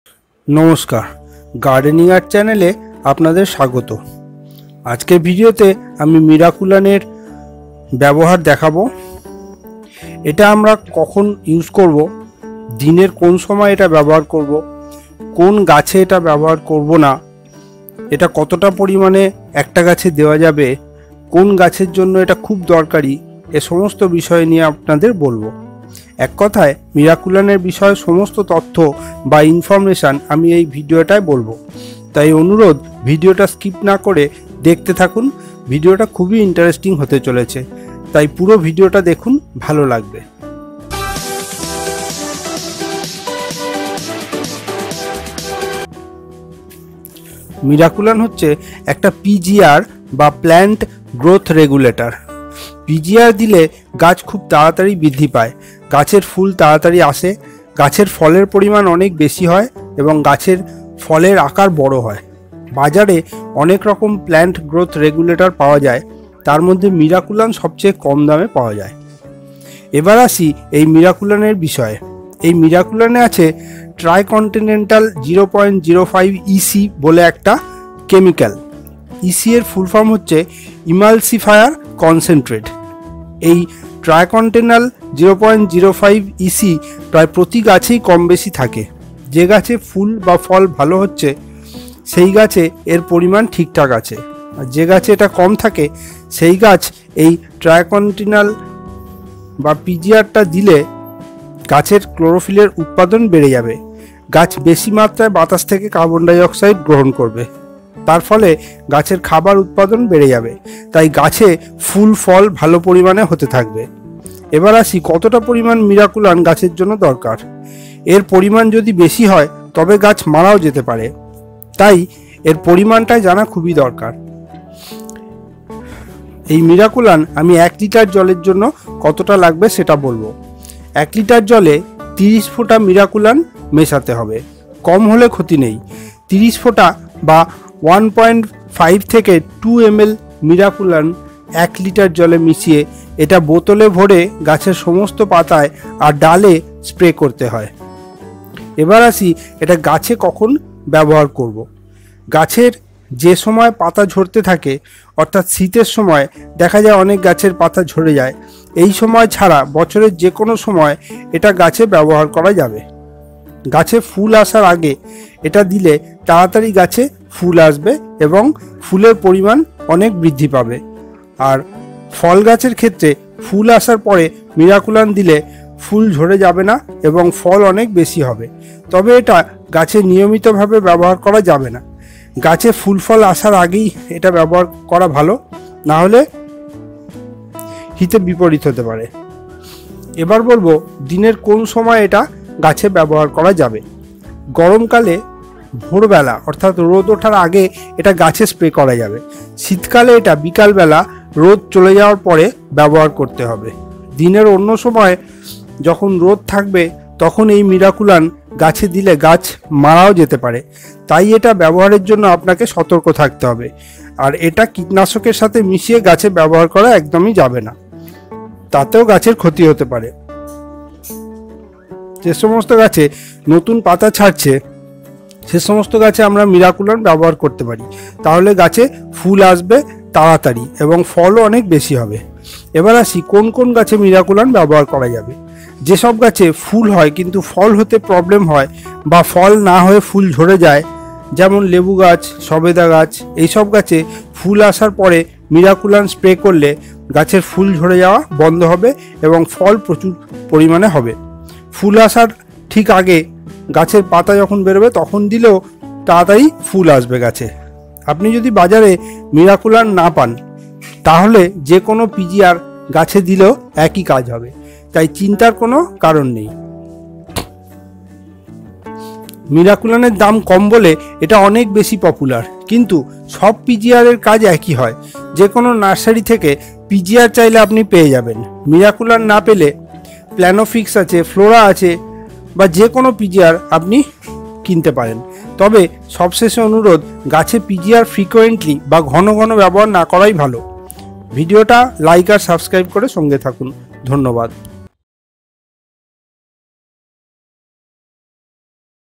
How to use Miraculan on plants | মিরাকুলান(Miraculan) কখন কিভাবে গাছে ব্যবহার করবেন | Gardening Art
How to use Miraculan on plants, মিরাকুলান কখন কিভাবে গাছে ব্যবহার করবেন